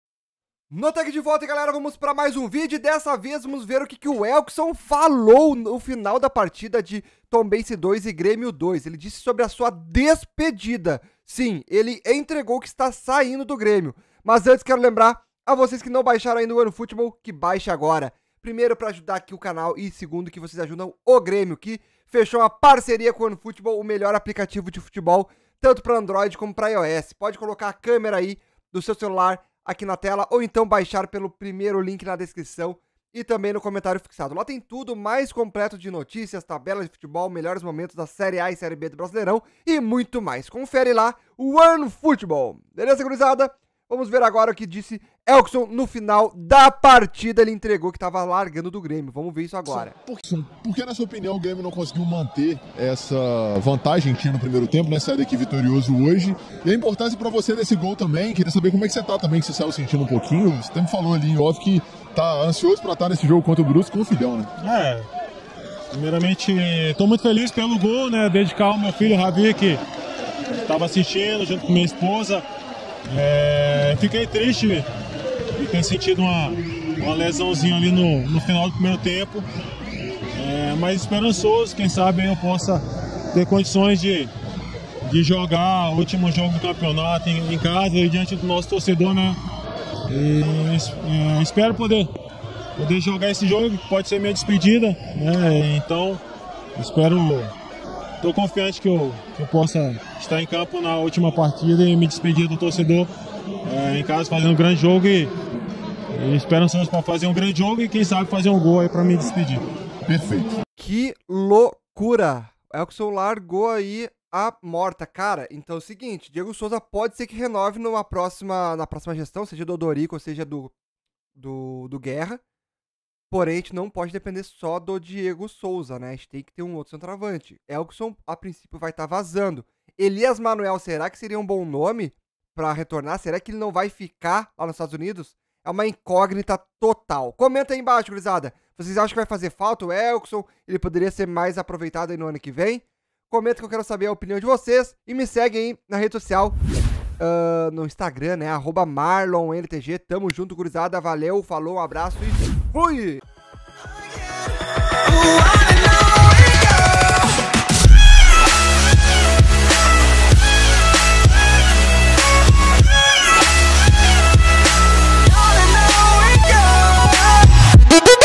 BOMBA! Veja o que ELKESSON falou no final do jogo, sobre sua DESPEDIDA!
Elkesson falou sobre despedida no final da partida contra o Tombense!
Imagens: Canal Premi3re